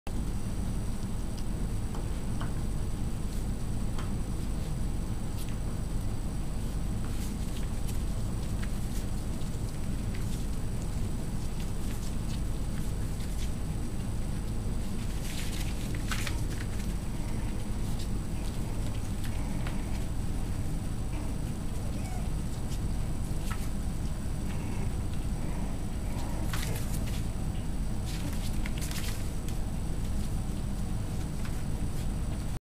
Hattie's wrestling.MOV
Schnauzer Puppies playing.  Hattie's 2011 summer litter